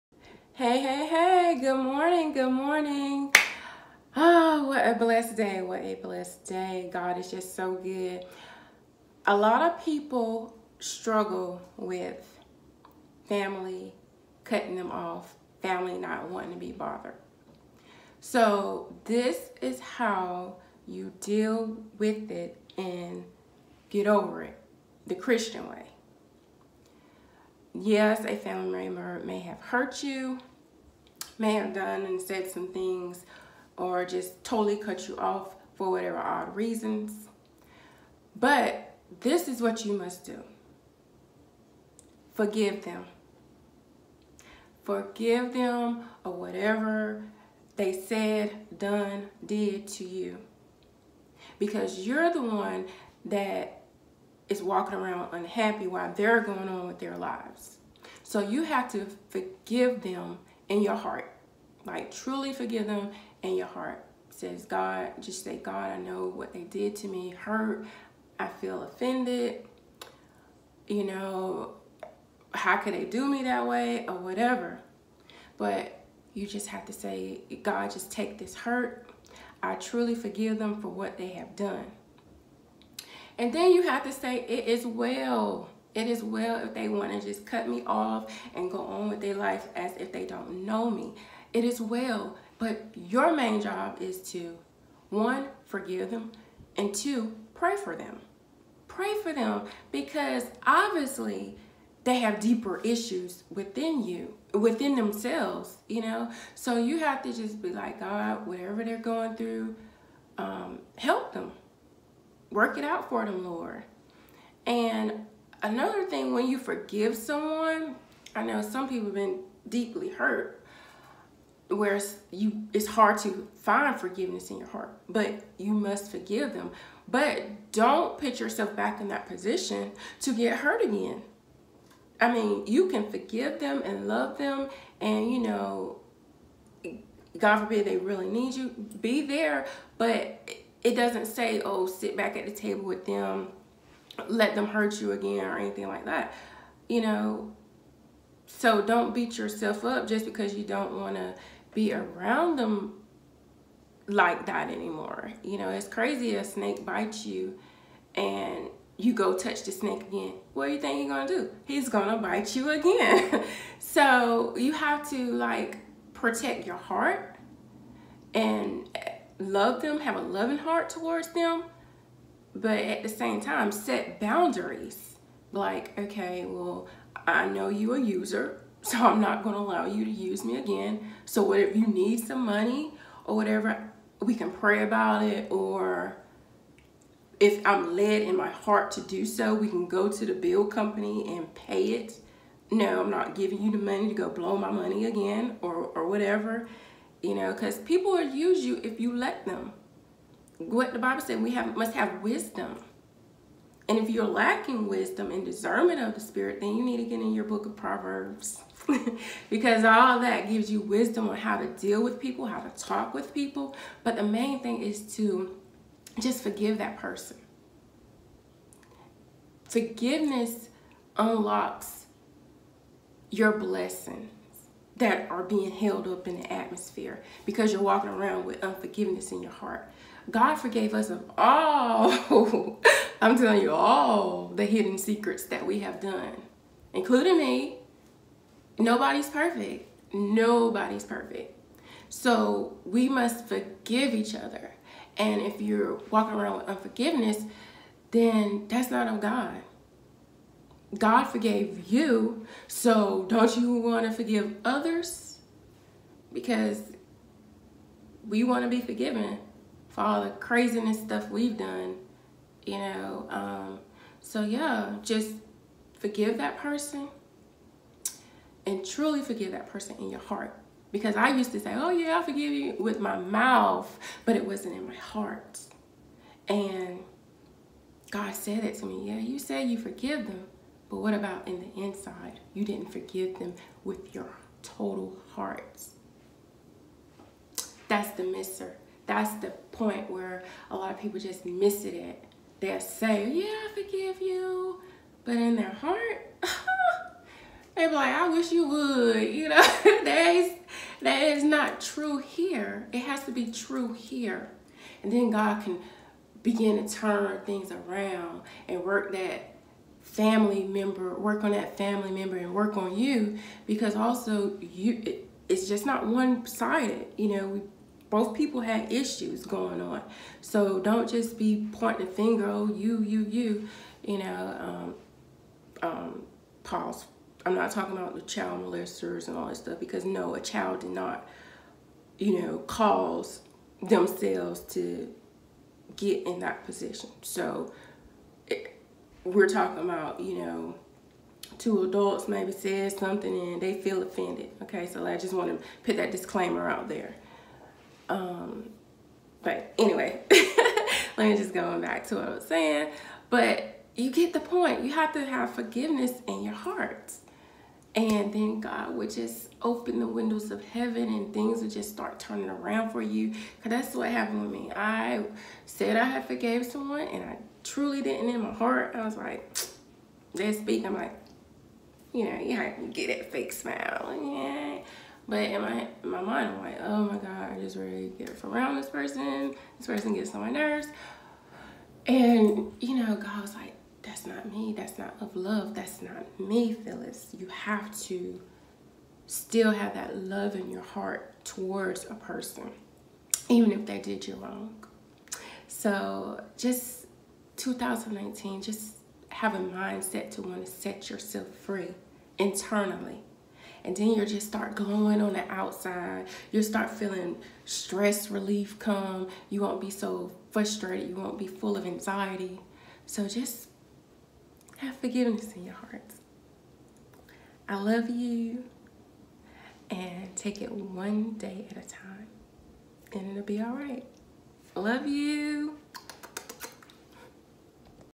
HOW TO GET OVER FAMILY HURT
Many of us have been hurt, disappointed or feel abandoned by our family, the closest people to us. But some times they feel the furthest away from us at the same time. Here’s how I got over my Family Hurt the Godly way.